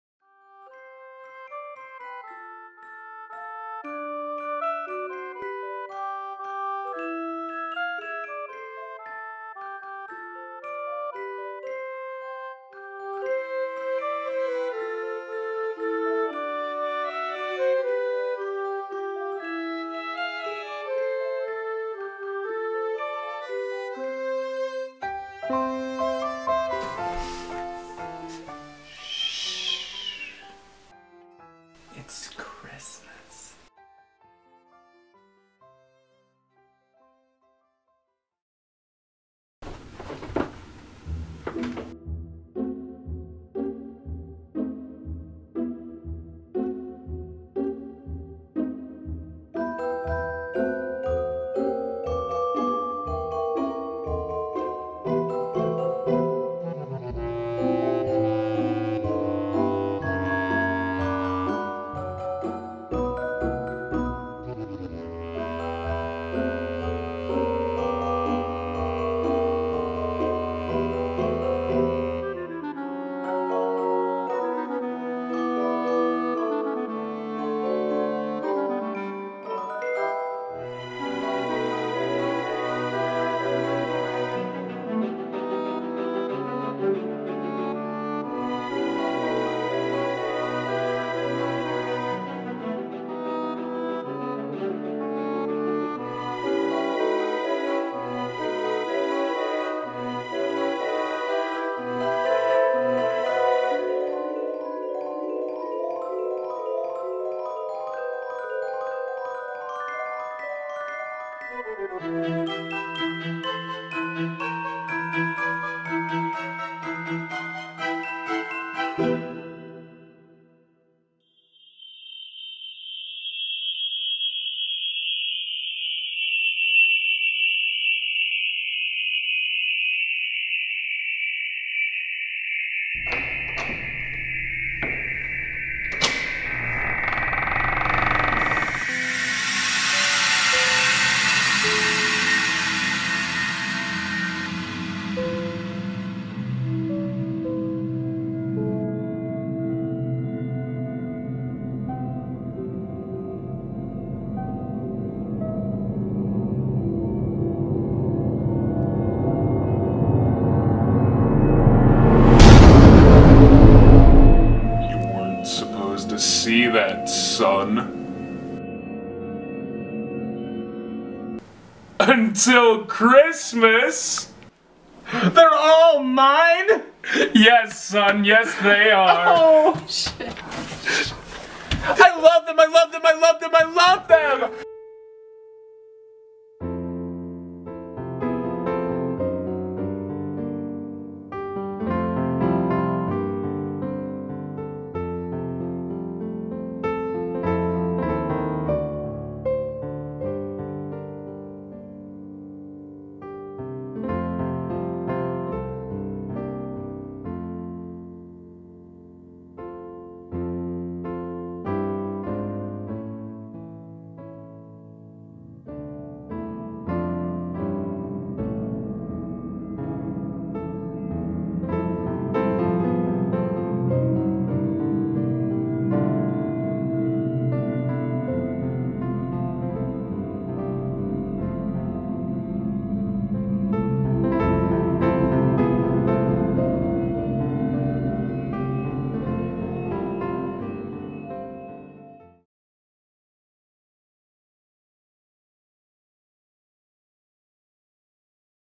Snooping for Christmas Presents (2013)
When a young boy wakes up early on Christmas to get a peak at his presents, he uncovers the greatest gift of all.

Originally posted on out predecessor Channel "Littlebootfeet"